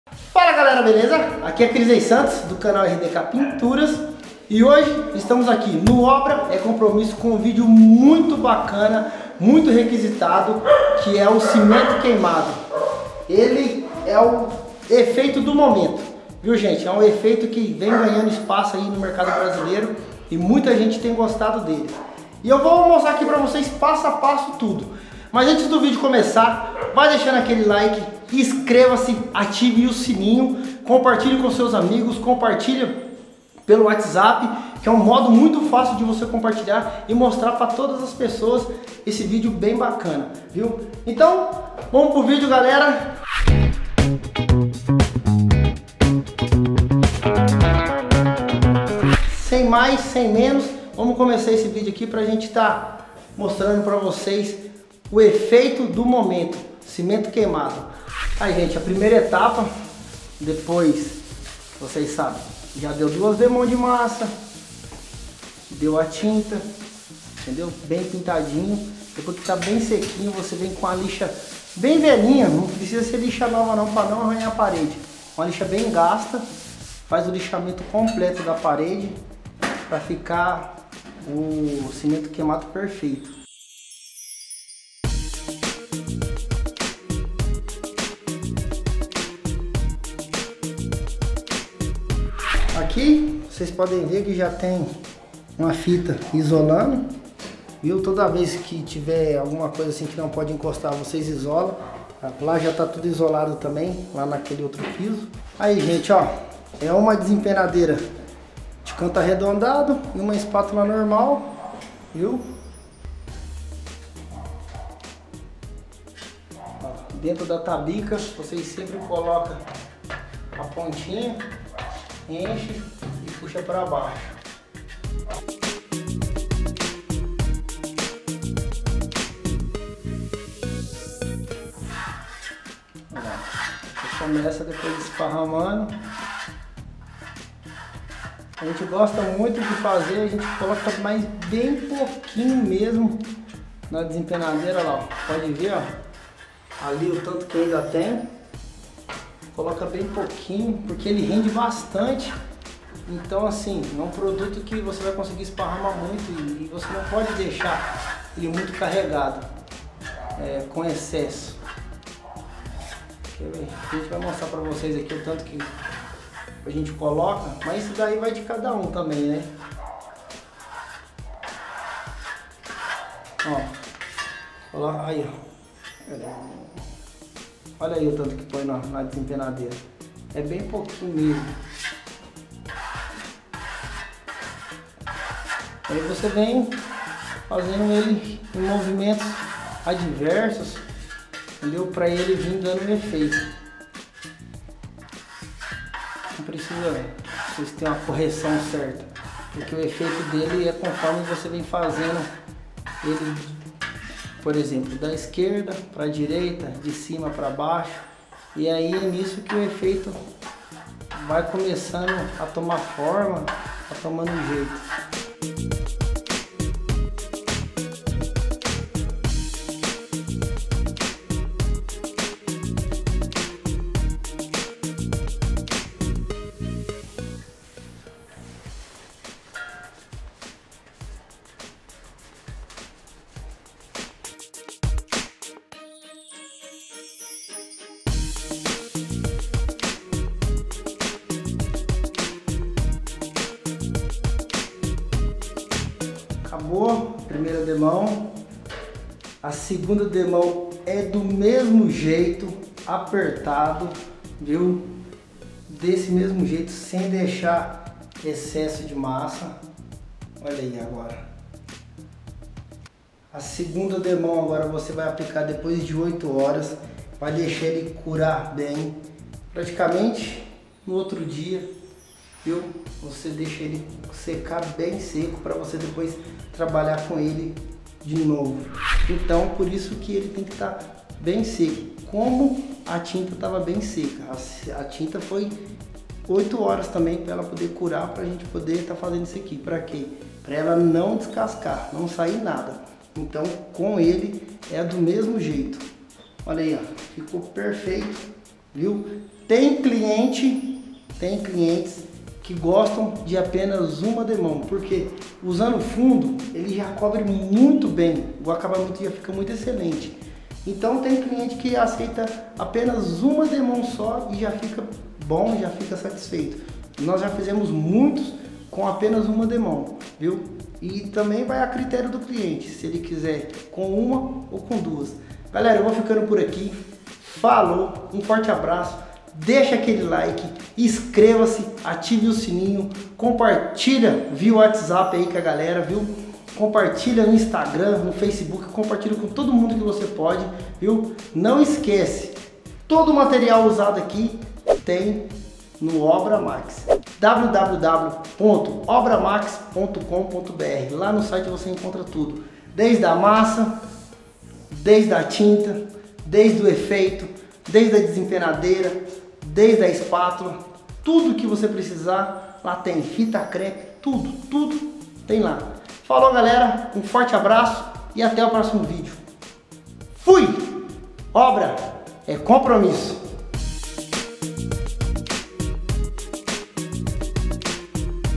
3 passos simples para aplicar cimento queimado do zero
Aprenda agora a aplicar cimento queimado passo a passo com Krisney Santos, embaixador aqui do canal Obra é Compromisso.

Após aplicar a massa corrida e a tinta na mesma cor do cimento queimado, pegue uma lixa gasta e lixe toda a parede. Aplique fita em todos encontros e objetos para protegê-los.

Se houver tabica na obra, não se esqueça de aplicar o cimento queimado antes dela também. Esparrame bem o cimento queimado, colocando pouco material na desempenadeira de canto arredondado e espalhando bem em movimentos assimétricos, que darão o efeito desejado.

Vá limpando a desempenadeira e a espátula ao longo do trabalho e aplique o cimento queimado em toda a parede. Se desejar, após 8 horas aplique uma segunda demão, lembrando sempre de não deixar excesso.